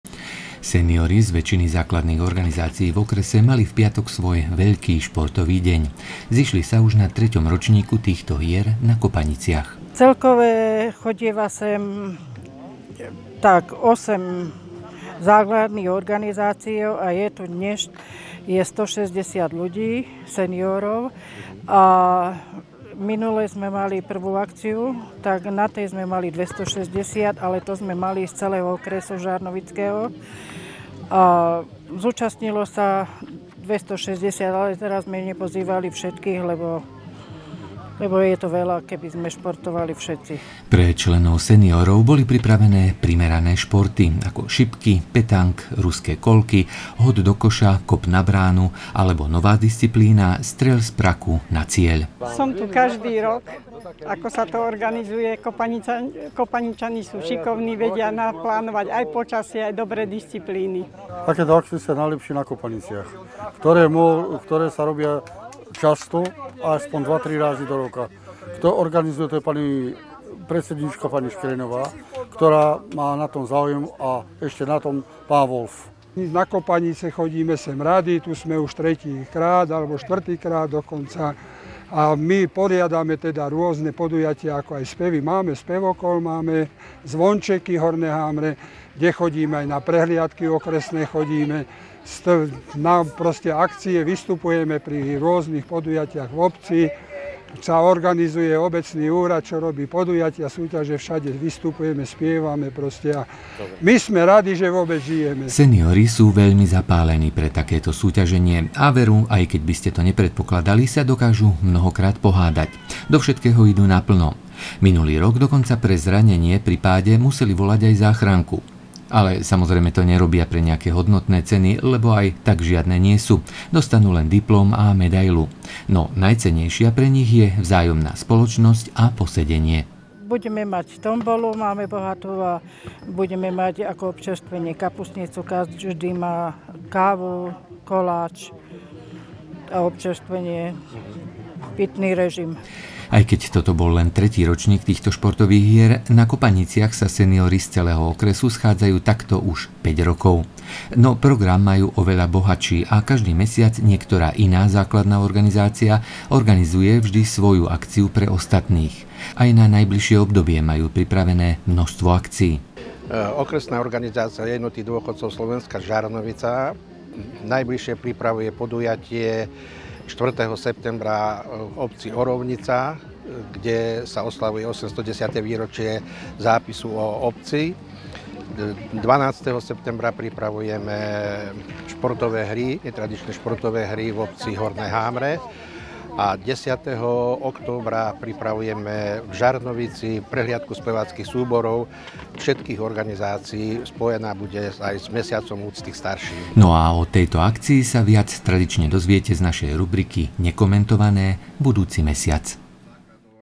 Minulý rok volali aj záchranku
Reportáž z ďalšieho ročníka športových hier seniorov na Kopaniciach.